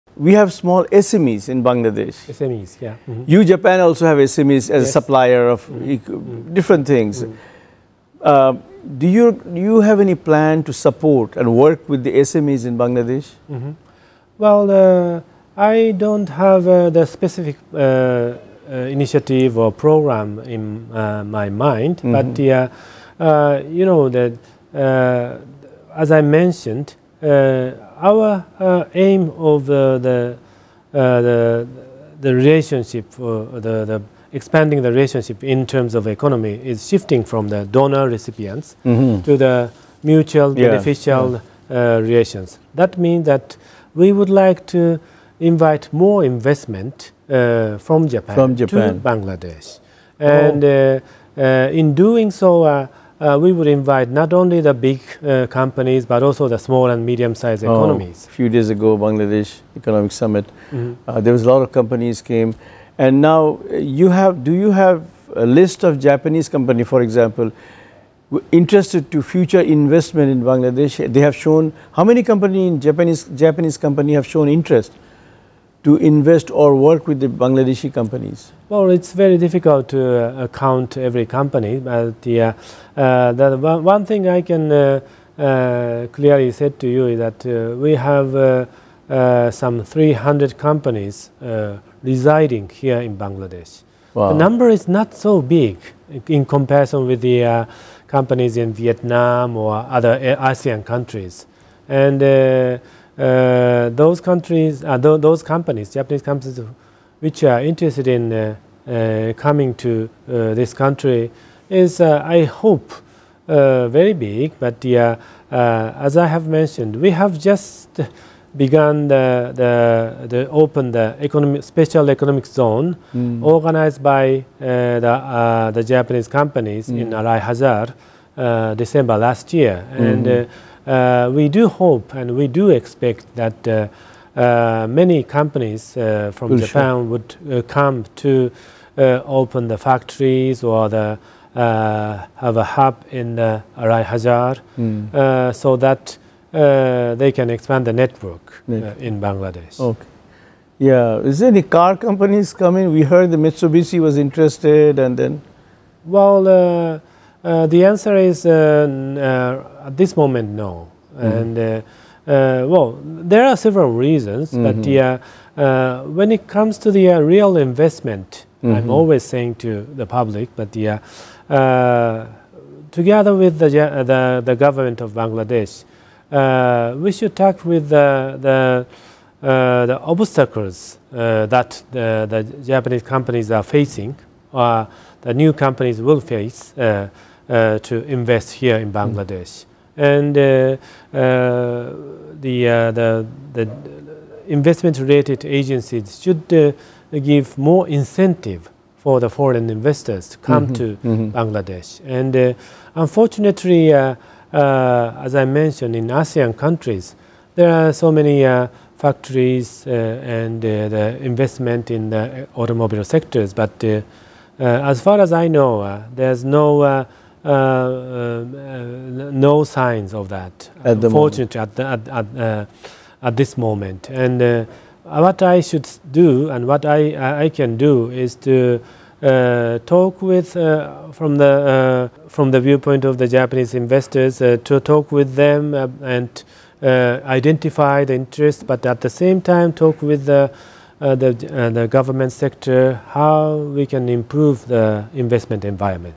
এসএমই ও গাড়ীর কারখানা নিয়ে যা বললেন জাপানি রাষ্টদূত || SME || Japanese ambassador || Deepto News
এসএমই ও গাড়ীর কারখানা নিয়ে যা বললেন জাপানি রাষ্টদূত

Address:
Deepto TV
7/A/GA Tejgaon Industrial Area,
Dhaka 1208, Bangladesh ||  || Deepto News